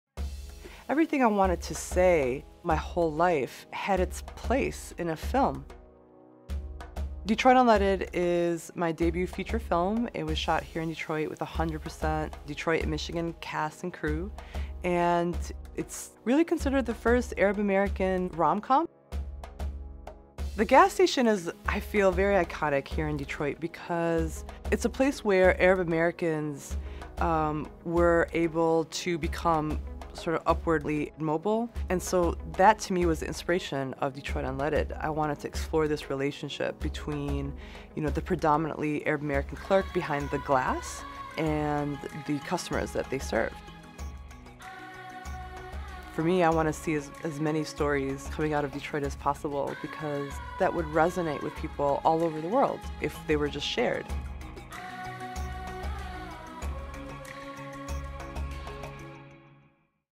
One Detroit Profile: Rola Nashef
Rola Nashef, Arab-American film director and producer, discusses her film, "Detroit Unleaded."